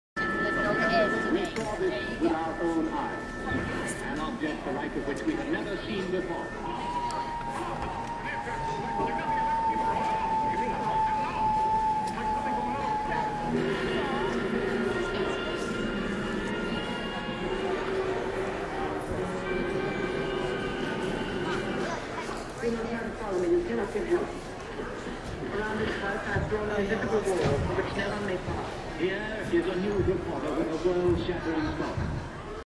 Sci Fi Drive In Dinner Theater - Hollywood Studios - Orlando, FL
This is where our son, Mason, wanted to eat for his 7th birthday.